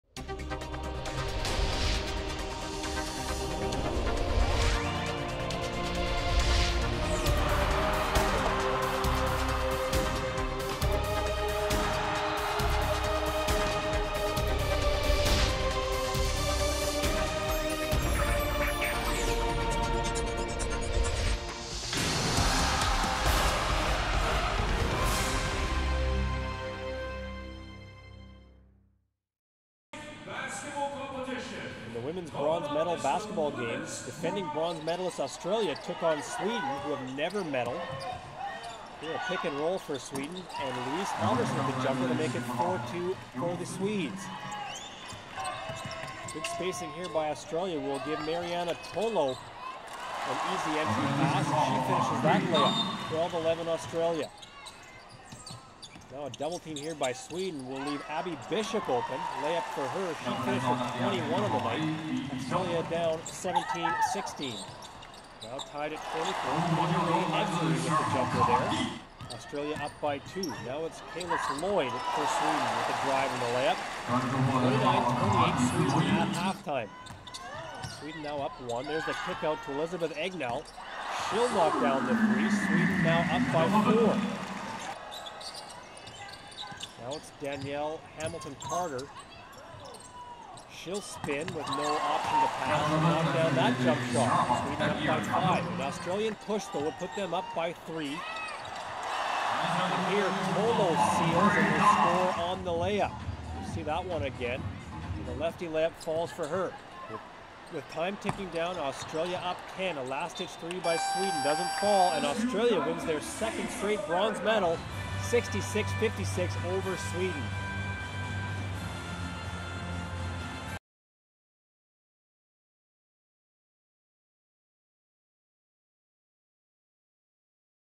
26th SU Shenzhen (CHN) - Basketball Women - Bronze medal
Bronze medals:  SWE 56  vs  AUS 66

BRONZE: AUS - Australia
4 GRAHAM Sarah
5 CUMMING Stephanie
6 BISHOP Abby
7 NEWLEY Mia
8 LANGFORD Emma
9 MADGEN Tess
10 HUNT Nicole
11 TOMLINSON Louella
12 EBZERY Katie Rae
13 PENALUNA Elyse
14 TOLO Marianna
15 FRANCIS Cayla